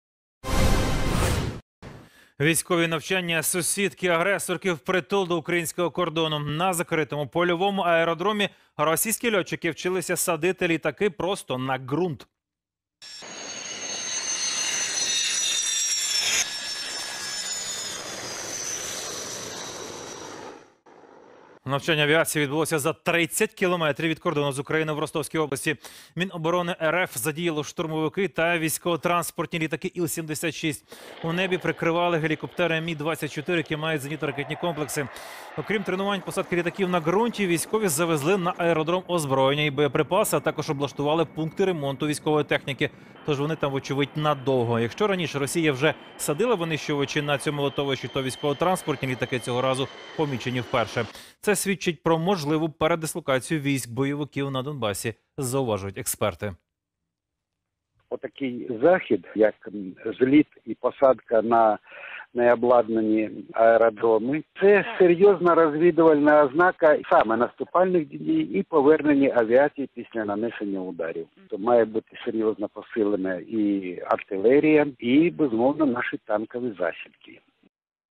Вблизи украинской границы замечены военно-транспортные самолеты ВКС РФ
Около украино-российской границы амечены военно-транспортные самолеты Военно-космических сил Российской Федерации, что может свидетельствовать о ротации российских боевиков на Донбассе